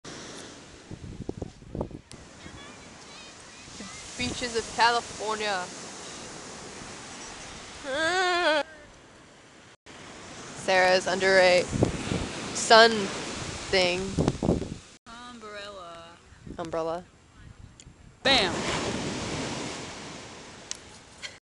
Sarah and Kat on The Beach
Sarah and Kat on The Beach in Santa Cruz. 2008.